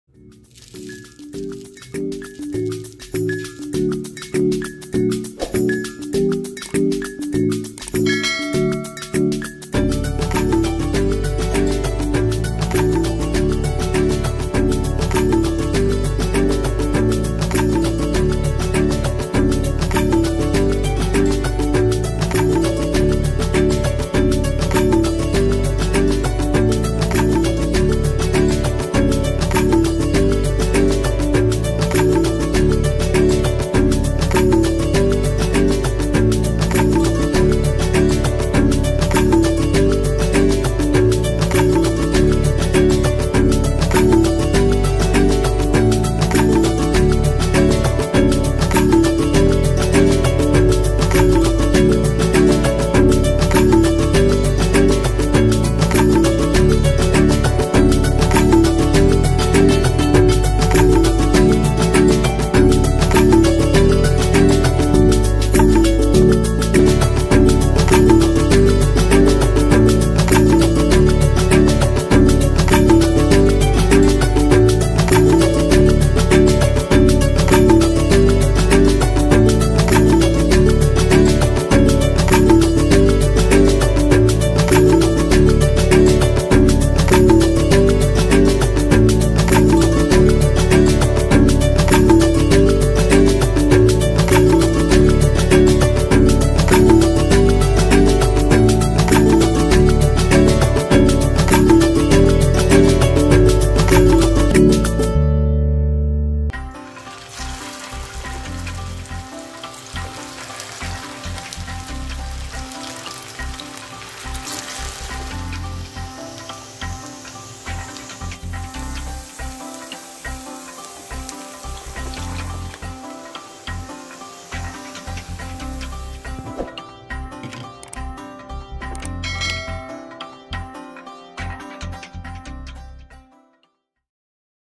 Aval Vada - Easy Crispy Savoury Snack || Flattened rice recipe || Poha/ Avalakki/ Atukulu Vada
Ingredients
Flattened rice 1/2 cup